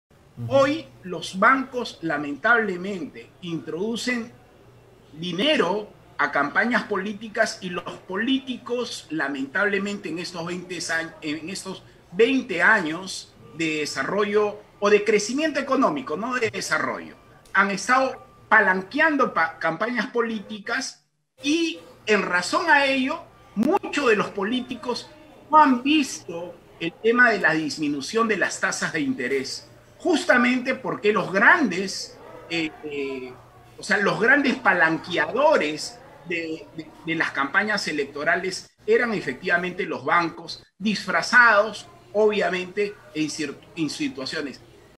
Bancos financian campañas electorales
🤝 Favor con favor se paga. Políticos no hacen nada ante las altas tasas de interés debido a que los bancos les financian sus campañas electorales, asegura Miguel Ángel Ferreyra, analista político.